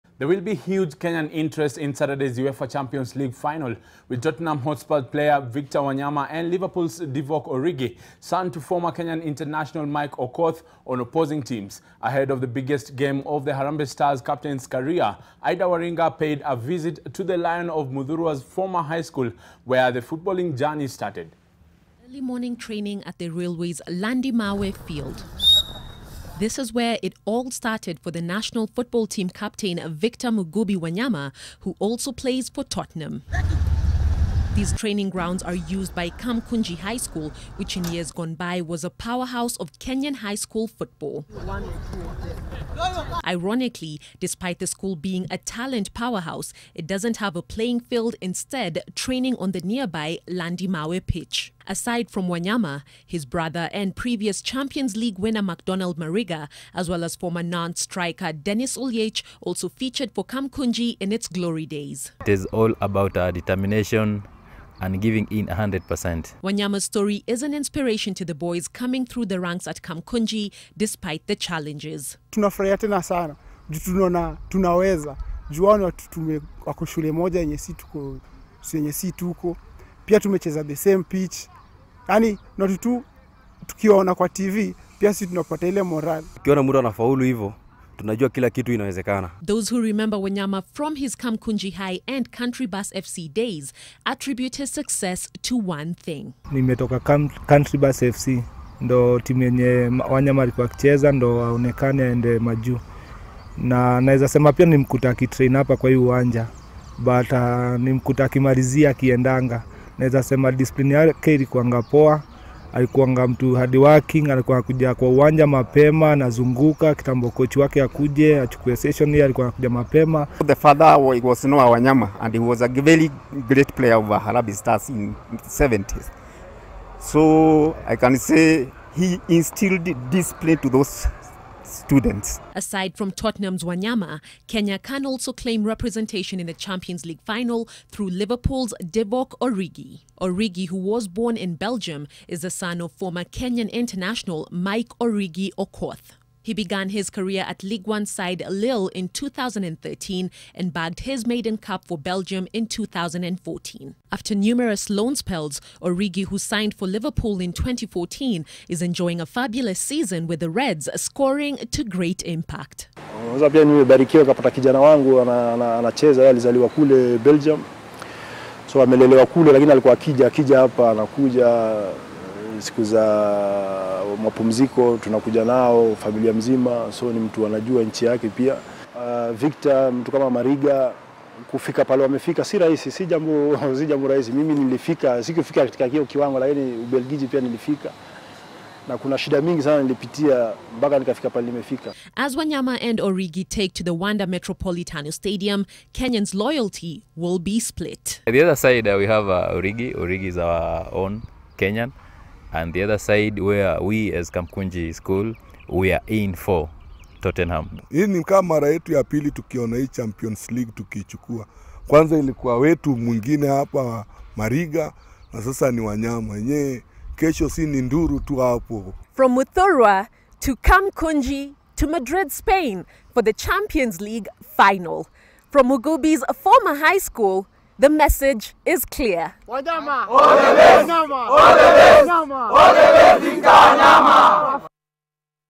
UEFA Champions League final: Wanyama to meet Origi, hope to match Mariga
There will be huge kenyan interest in saturday's uefa champions league final with tottenham hotspur player victor wanyama and liverpool's divock origi, son to former kenyan International Mike Okoth, on opposing teams.

Get the news updates, discussions and other exciting shows.